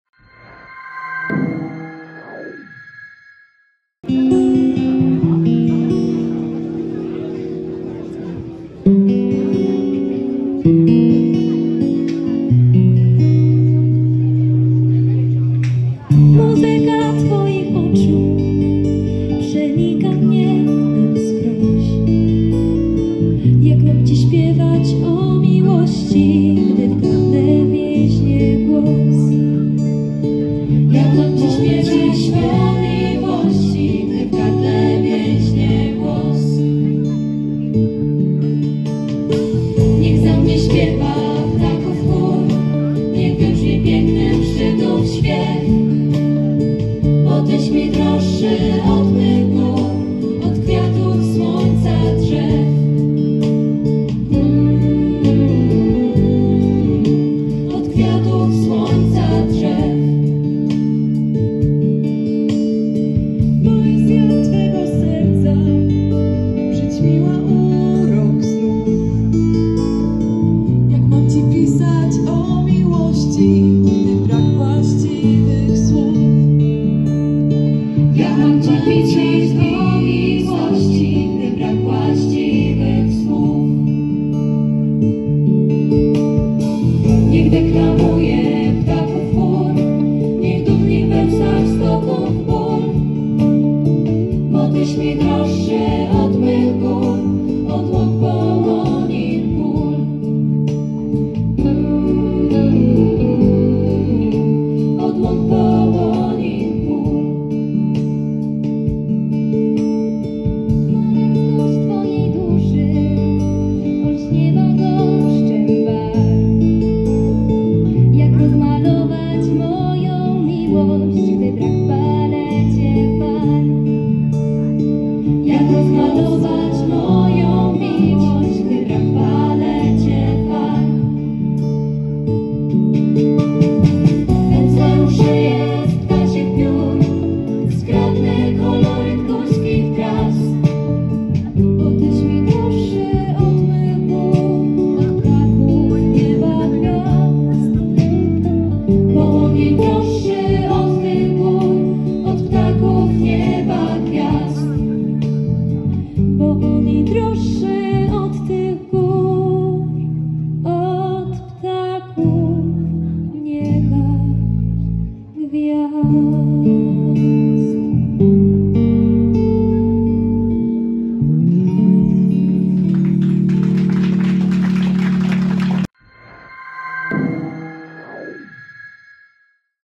Koncert zespołu "Wołosatki" w Zatwarnicy
Koncert zespołu "Wołosatki" podczas Kiermaszu Bojkowskiego w Zatwarnicy.

wBieszczady.pl